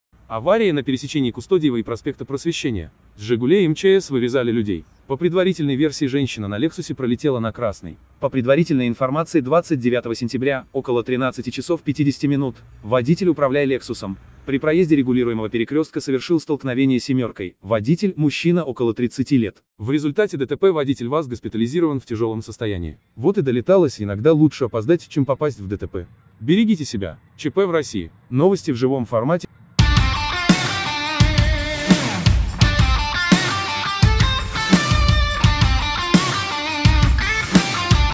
Новости ДТП СПБ - Женщина на Лексусе разнесла Жигули ! 😱
Авария на пересечении Кустодиева и проспекта Просвещения. С Жигулей МЧС  вырезали людей. По предварительной версии женщина на Лексусе пролетела на красный. По предварительной информации 29 сентября, около 13:50, водитель  управляя Лексусом, при проезде регулируемого перекрестка совершил столкновение семеркой (водитель - мужчина около 30 лет). В результате ДТП водитель ВАЗ госпитализирован в тяжелом состоянии. Вот и до литалась, иногда лучше опоздать, чем попасть в дтп. Берегите себя.